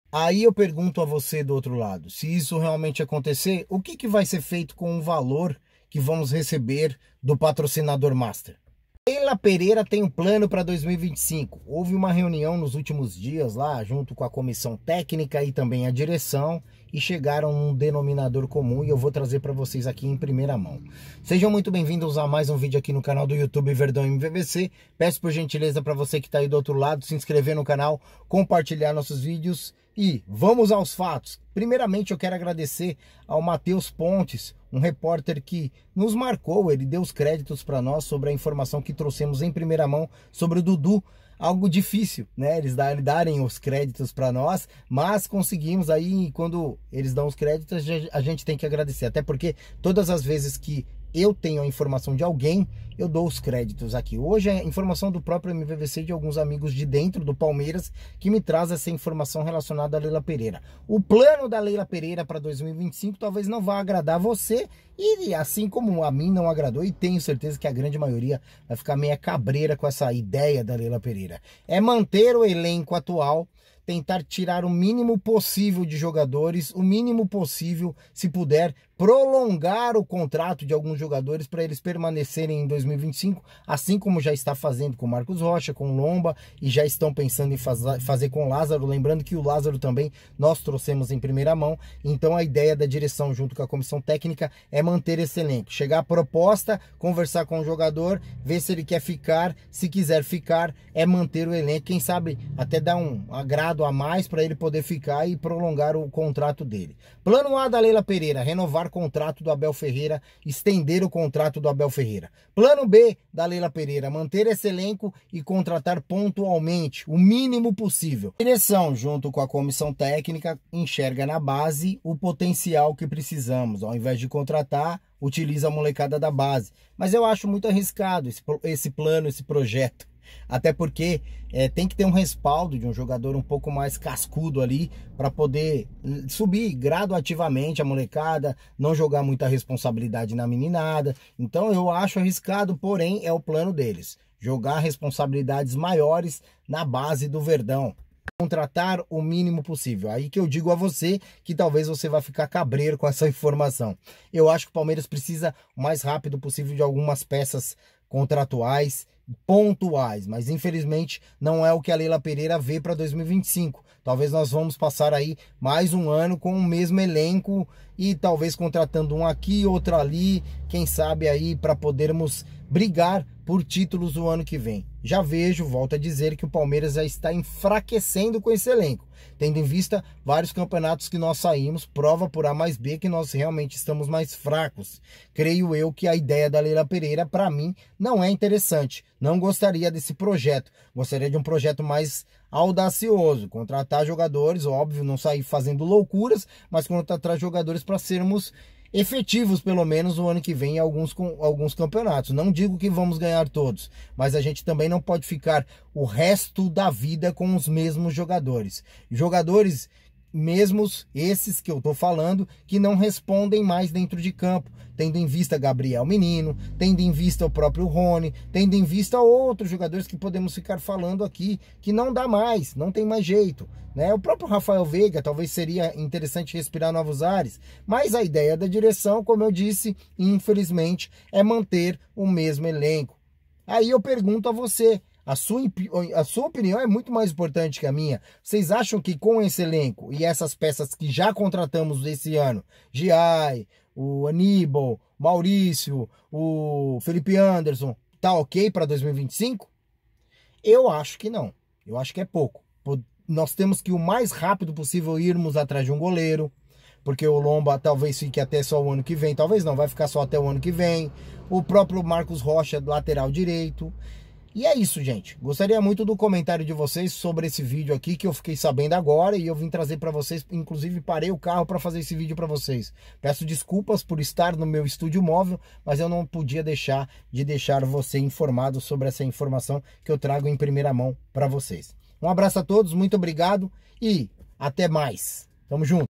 💣🔥BOMBA! ESSA IDEIA DA LEILA PEREIRA PRA 2025 PREOCUPA.
✅ Informação
A ideia da Leila é do Anderson Barros é manter o elenco atual e contratar o mínimo possível para 2025.
Renovação contratual não está descartada de vários jogadores para manter  e prolongar o vínculo com o clube.
Plano 1: Renovar com Abel
Plano 2: Manter o mesmo elenco para 2025.

Canal do YouTube Verdão Mvvc